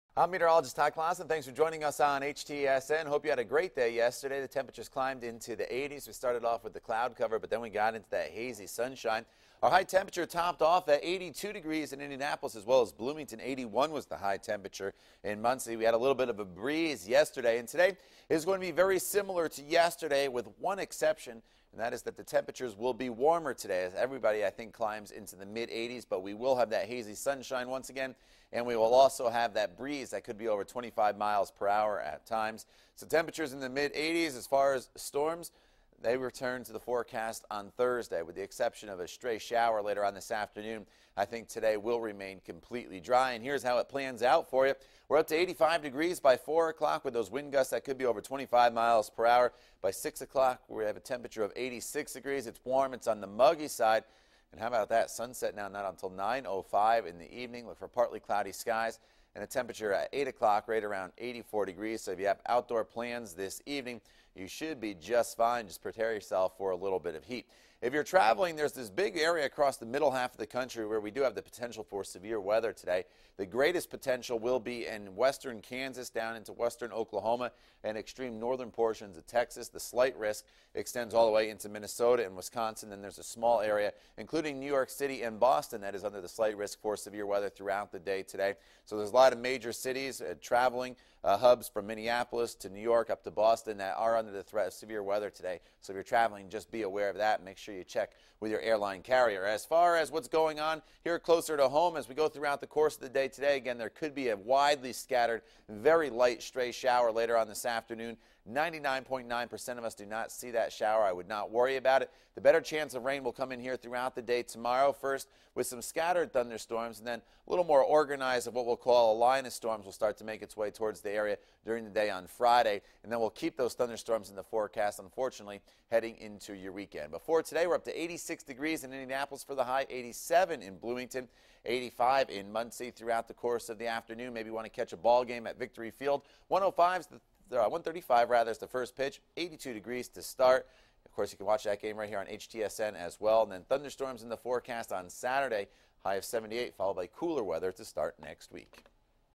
Wedensday's Forecast: Warm, breezy with partly cloudy skies
Wednesday will be warm, breezy with partly cloudy skies and highs in the 80s.